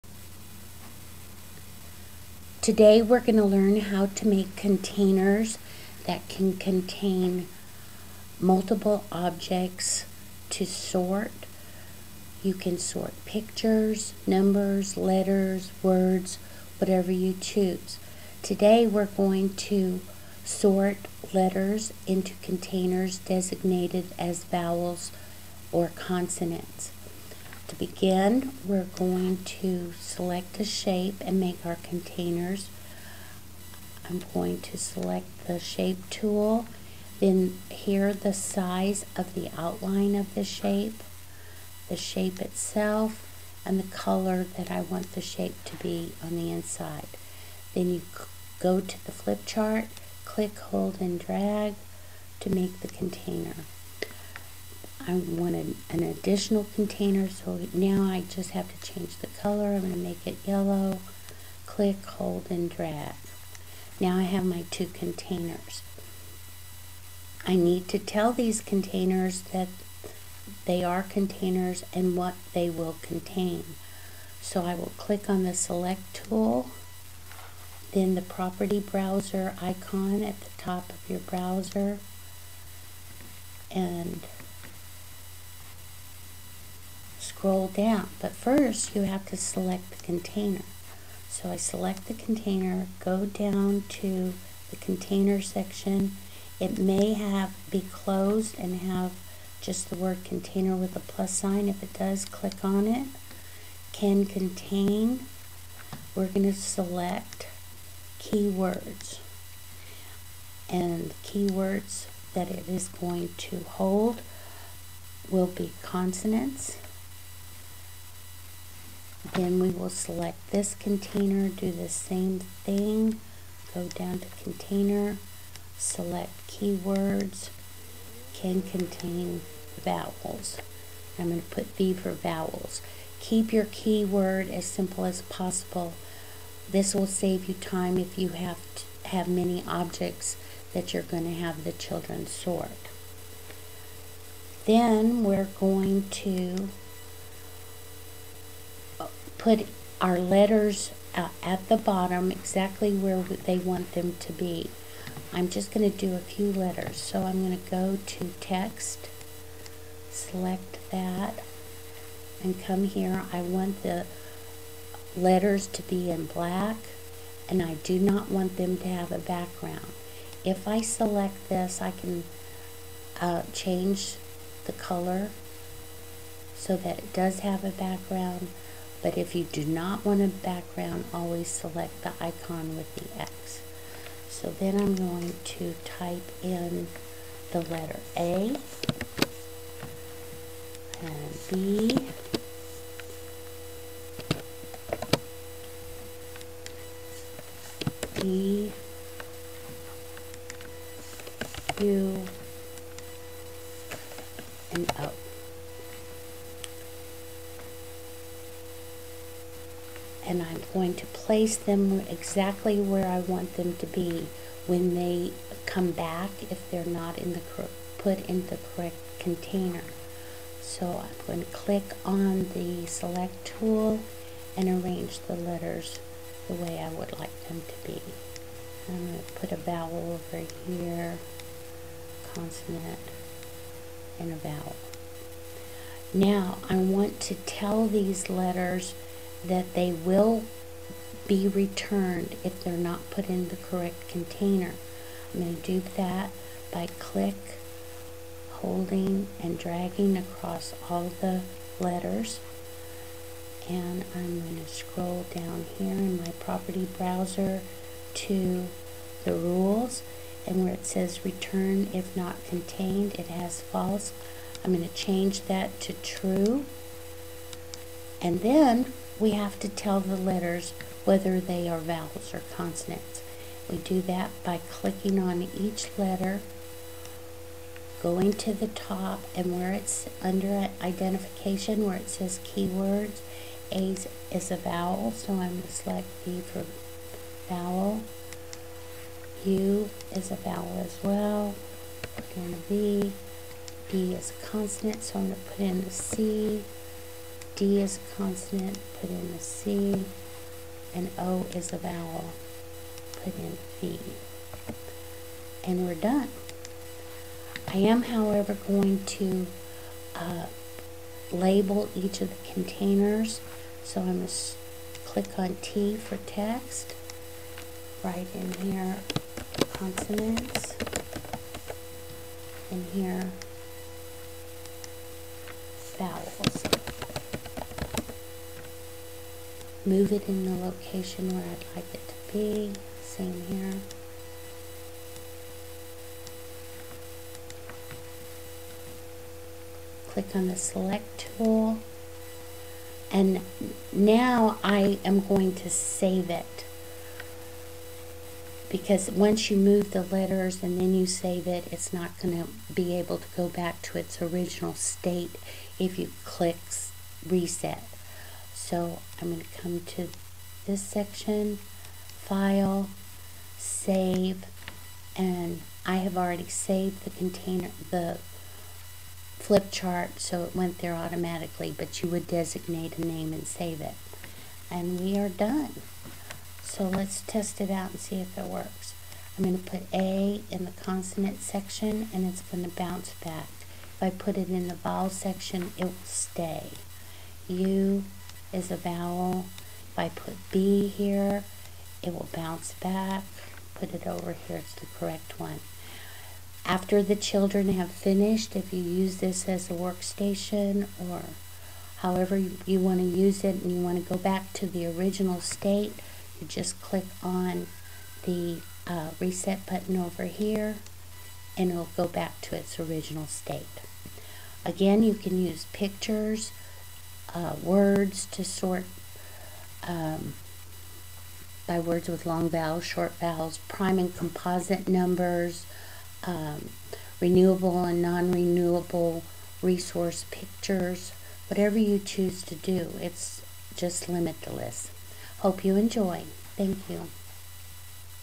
Promethean Containers for Keywords
This video will show you how to contain multiple objects or words using Promethean Inspire software.